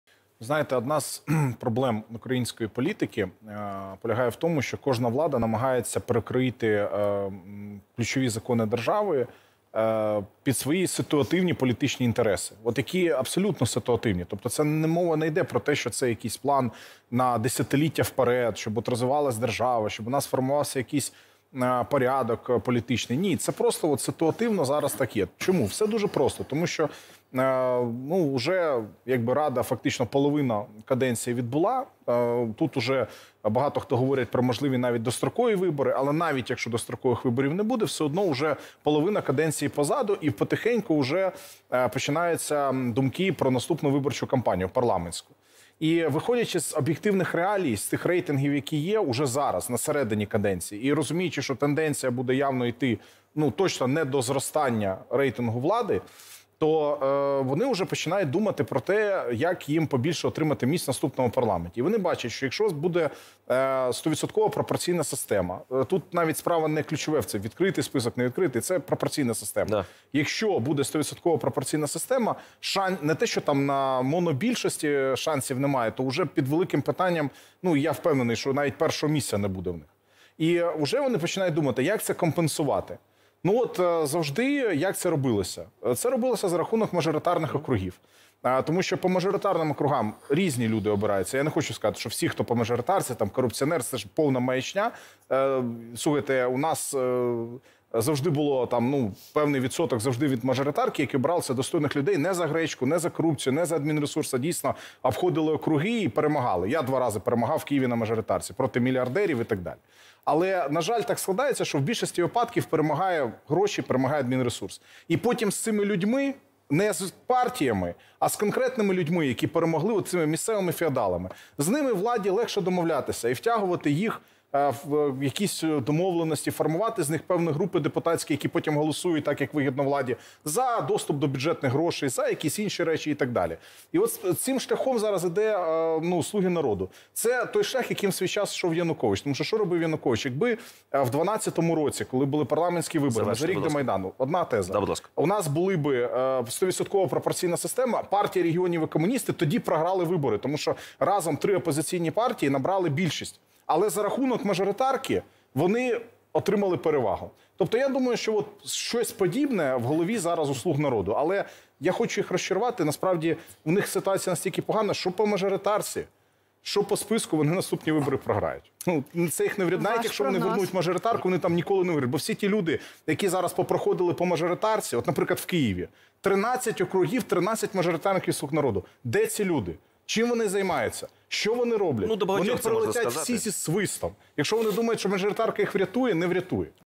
У «Слуг народу» настільки погана ситуація, що їх не врятує і зміна виборчої системи — Андрій Іллєнко
Одна з ключових проблем української політики полягає у тому, що кожна влада намагається перекроїти ключові закони держави під свої ситуативні політичні інтереси. "Слуги народу" йдуть шляхом Партії Регіонів. Але в них настільки погана ситуація, що їх не врятує і зміна виборчої системи, — Андрій Іллєнко, заступник голови ВО "Свобода" в етері телеканалу "Україна 24".